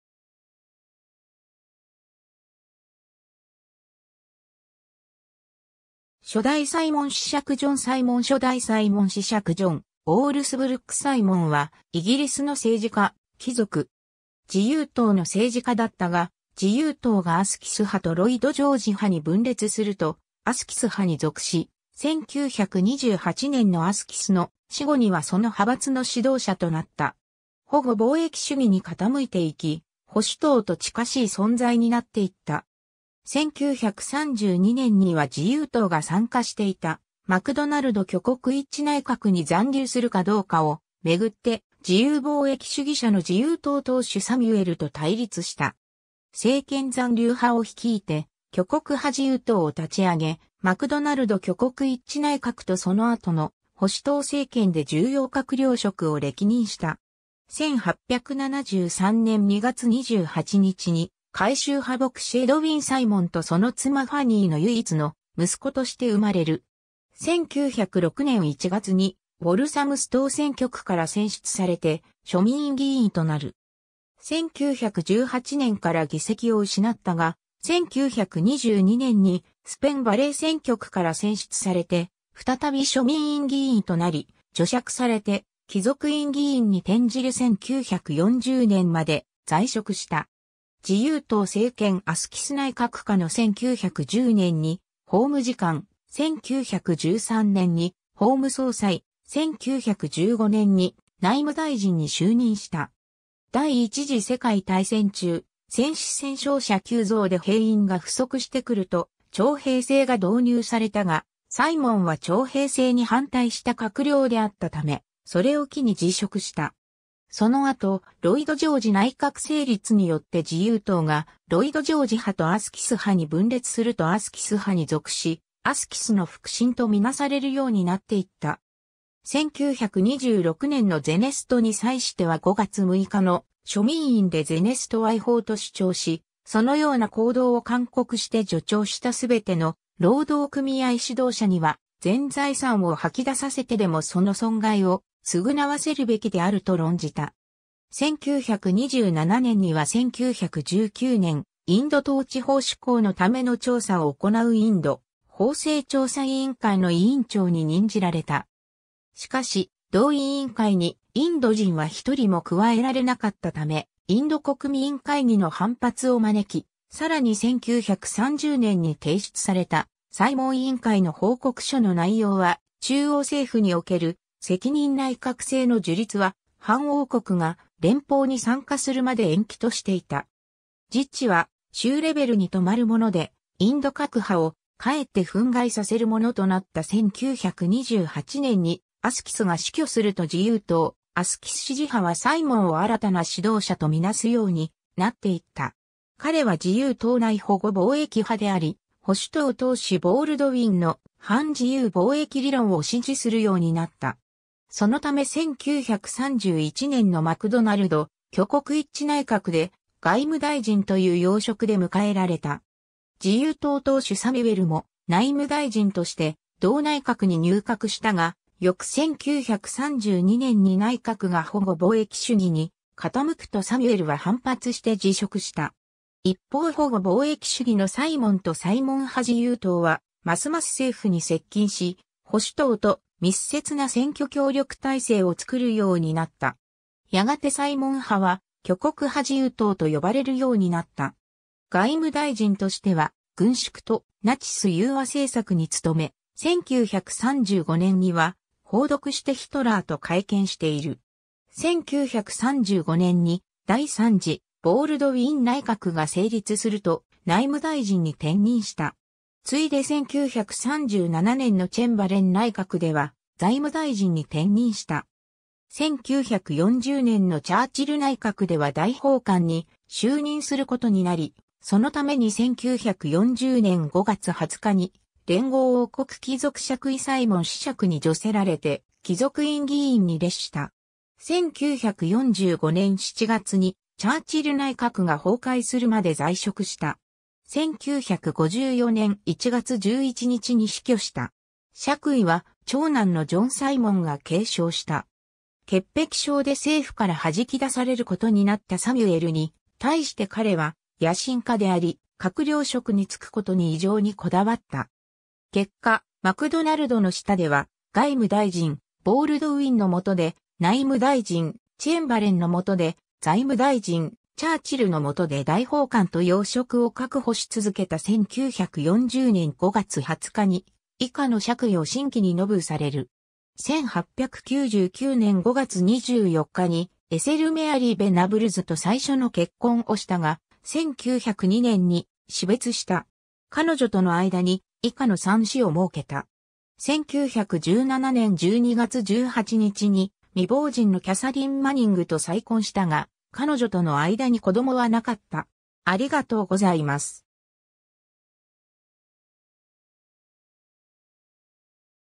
ジョン・サイモン (初代サイモン子爵)
初代サイモン子爵ジョン・サイモン 初代サイモン子爵ジョン・オールスブルック・サイモン（英: John Allsebrook Simon, 1st Viscount Simon, GCSI, GCVO, OBE, PC、1873年2月28日 - 1954年1月11日）は、イギリスの政治家・貴族。
 自由党の政治家だったが、自由党がアスキス派とロイド・ジョージ派に分裂するとアスキス派に属し、1928年のアスキスの死後にはその派閥の指導者となった。
保護貿易主義に傾いていき、保守党と近しい存在になっていった。
1932年には自由党が参加していたマクドナルド挙国一致内閣に残留するかどうかを巡って自由貿易主義者の自由党党首サミュエルと対立した。
政権残留派を率いて挙国派自由党を立ち上げ、マクドナルド挙国一致内閣とその後の保守党政権で重要閣僚職を歴任した。
 1873年2月28日に会衆派牧師エドウィン・サイモンとその妻ファニー（旧姓オールスブルック）の唯一の息子として生まれる。
 1906年1月にウォルサムストー選挙区から選出されて庶民院議員となる。
1918年から議席を失ったが、1922年にスペン・バレー選挙区から選出されて再び庶民院議員となり、叙爵されて貴族院議員に転じる1940年まで在職した。
 自由党政権アスキス内閣下の1910年に法務次官、1913年に法務総裁、1915年に内務大臣に就任した。
 第一次世界大戦中、戦死戦傷者急増で兵員が不足してくると徴兵制が導入されたが、サイモンは徴兵制に反対した閣僚であったため、それを機に辞職した。
その後ロイド・ジョージ内閣成立によって自由党がロイド・ジョージ派とアスキス派に分裂するとアスキス派に属し、アスキスの腹心と見做されるようになっていった。
 1926年のゼネストに際しては5月6日の庶民院でゼネストは違法と主張し、「そのような行動を勧告して助長したすべての労働組合指導者には全財産を吐き出させてでもその損害を償わせるべきである」と論じた。
 1927年には1919年インド統治法施行のための調査を行うインド法制調査委員会（サイモン委員会）の委員長に任じられた。
しかし同委員会にインド人は一人も加えられなかったため、インド国民会議の反発を招き、さらに1930年に提出されたサイモン委員会の報告書の内容は中央政府における責任内閣制の樹立は藩王国が連邦に参加するまで延期としていた（藩王国が連邦に参加する見込みはなかったので実質的に無期限延期）。
自治は州レベルに留まるものでインド各派をかえって憤慨させるものとなった 1928年にアスキスが死去すると自由党アスキス支持派はサイモンを新たな指導者と見做すようになっていった。
彼は自由党内保護貿易派であり、保守党党首ボールドウィンの反自由貿易理論を支持するようになった。
そのため1931年のマクドナルド挙国一致内閣（実質的には保守党主導の政権）で外務大臣という要職で迎えられた。
自由党党首サミュエルも内務大臣として同内閣に入閣したが、翌1932年に内閣が保護貿易主義に傾くとサミュエルは反発して辞職した。
一方保護貿易主義のサイモンとサイモン派自由党（約35名）はますます政府に接近し、保守党と密接な選挙協力体制を作るようになった。
やがてサイモン派は挙国派自由党と呼ばれるようになった。
外務大臣としては軍縮とナチス融和政策に努め、1935年には訪独してヒトラーと会見している。
 1935年に第三次ボールドウィン内閣が成立すると内務大臣に転任した。
ついで1937年のチェンバレン内閣では財務大臣に転任した。
1940年のチャーチル内閣では大法官（貴族院議長）に就任することになり、そのために1940年5月20日に連合王国貴族爵位サイモン子爵に叙せられて、貴族院議員に列した。
1945年7月にチャーチル内閣が崩壊するまで在職した。
 1954年1月11日に死去した。
爵位は長男のジョン・サイモンが継承した。
 潔癖症で政府からはじき出されることになったサミュエルに対して彼は野心家であり、閣僚職に就くことに異常にこだわった。
結果マクドナルドの下では外務大臣、ボールドウィンの下で内務大臣、チェンバレンの下で財務大臣、チャーチルの下で大法...